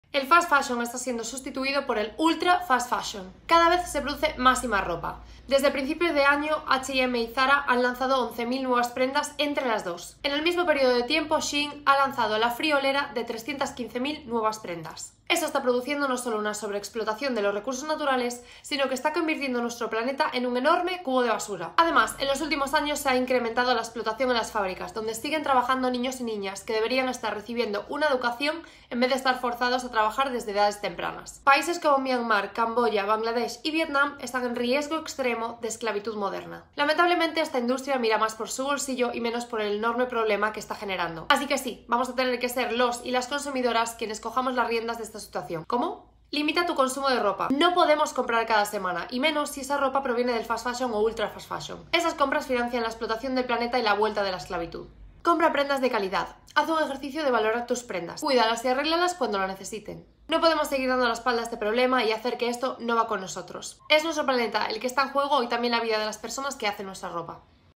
El FAST FASHION está siendo sustituido por el ULTRA FAST FASHION ❌😱
Es aterrador el panorama en que nos encontramos con respecto a la industria de la moda. Esta forma de producción MASIVA y súper RÁPIDA tiene que PARAR ✋🏼🛑

Empezando por nuestros hábitos de consumo. Lxs que compráis en SHEIN teniendo oportunidad de comprar en otros sitio, por favor, dejad de consumir ultra fast fashion 🙏🏻 Realmente está destrozando nuestro planeta y vidas de personas en países más pobres. Hagamos todxs un ejercicio de empatía y dejemos de dar dinero a empresas que explotan gente y recursos naturales 🌿